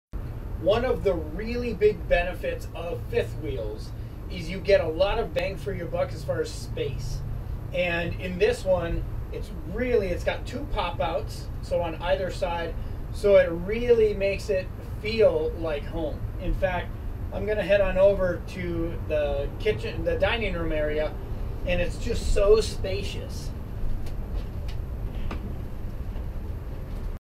5th Wheels are Spacious
With a double Popout a 5th wheel or Toy Hauler can become wider than many homes.